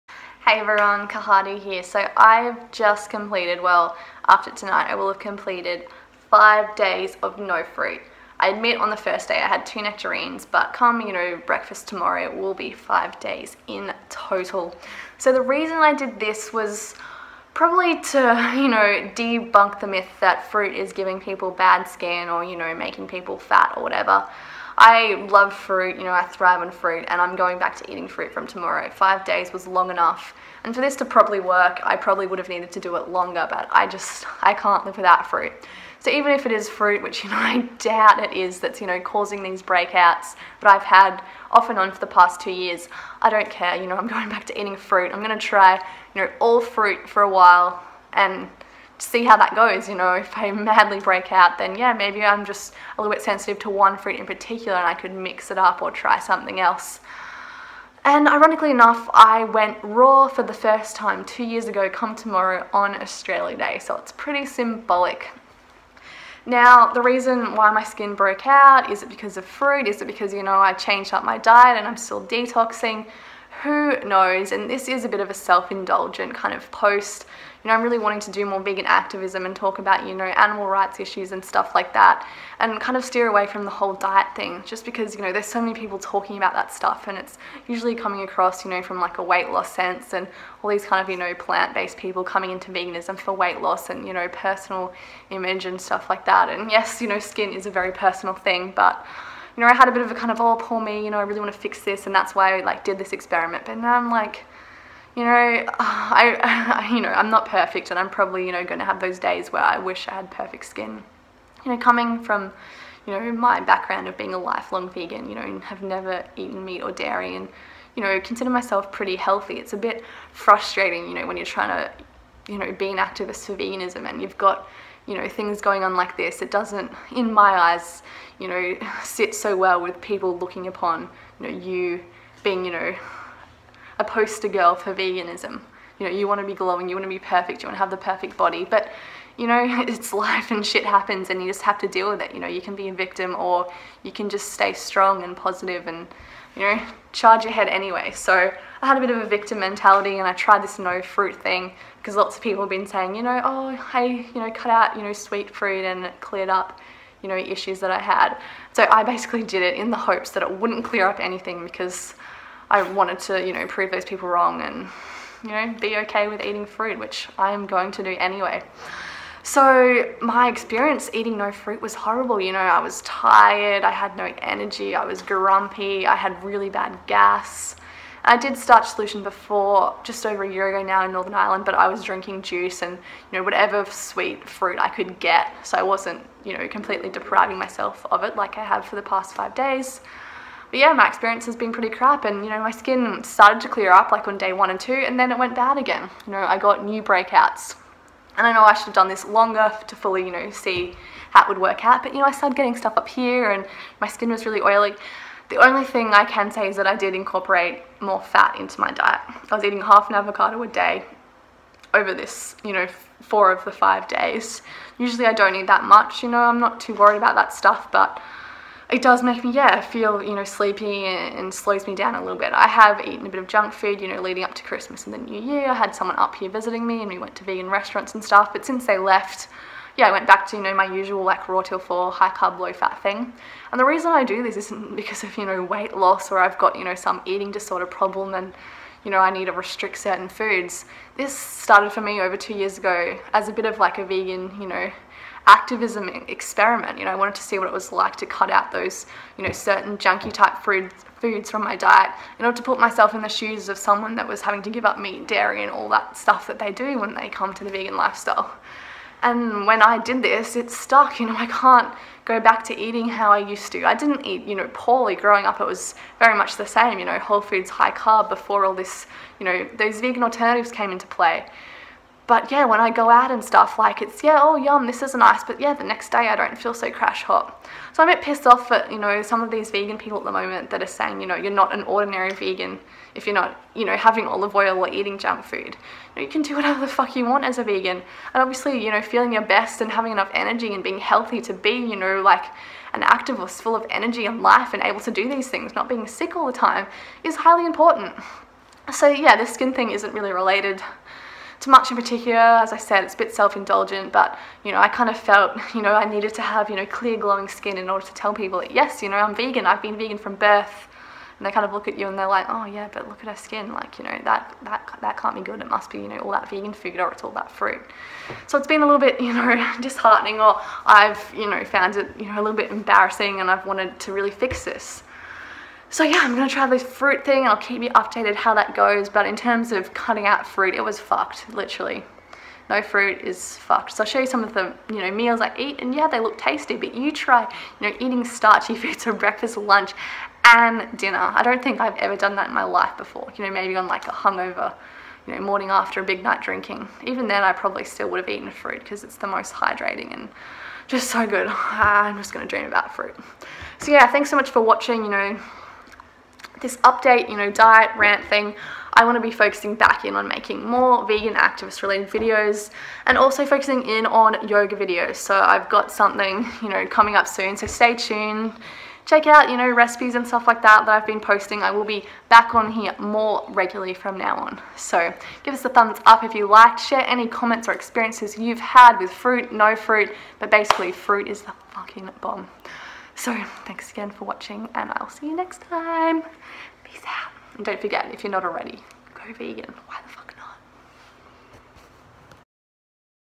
Acne from HighCarb Vegan Diet!? My NO FRUIT Experiment
I've been experiencing skin breakouts over the past two years since transiting to the high-carb low-fat vegan diet from a more normal vegan lifestyle my entire life.
It started with my first banana island and since has since come and gone (I had clear skin for the 4 months I was overseas in India and Thailand April-Aug 2015). I love fruit too much to live without it for most of my daily food intake, so I'll go on fruit only and then add back in my RawTill4 style dinners seeing how it goes with zero fat for awhile!

PEACE, KALE, BANANAS & LIVE VEGAN :) Xx